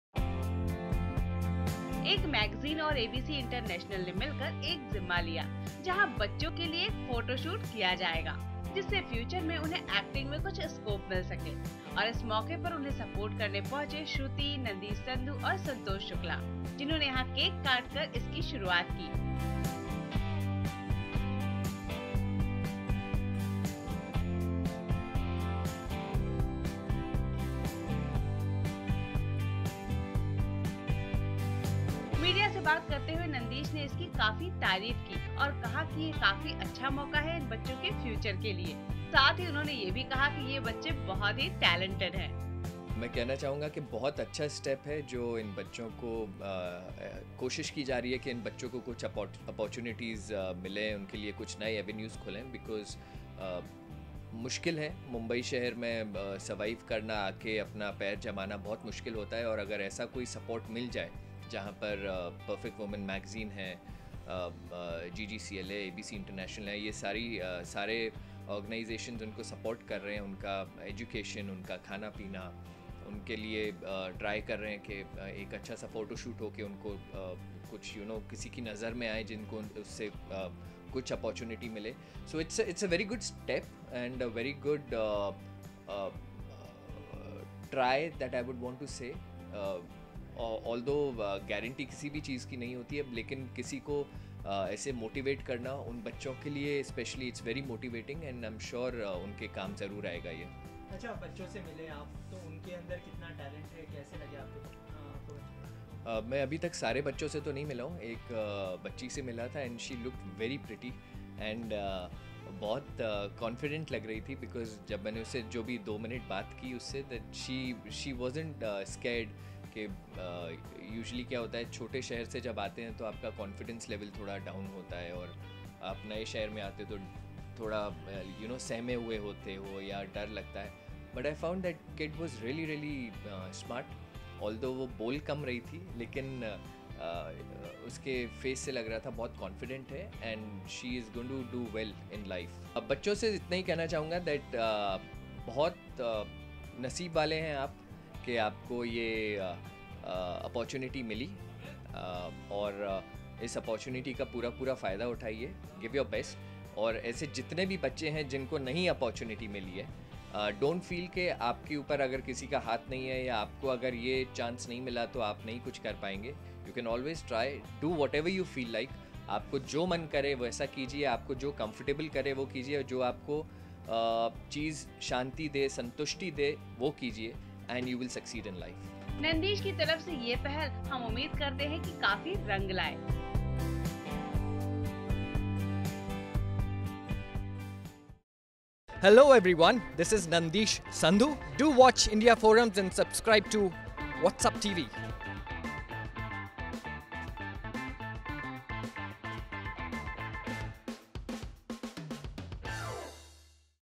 Nandish Sandhu Supports A Noble Cause For Orphanage The Kids
Watch the popular actors Nandish Sandhu,Shruti Bapna and Santosh Shukla supporting a noble cause to secure the future of Orphanage kids.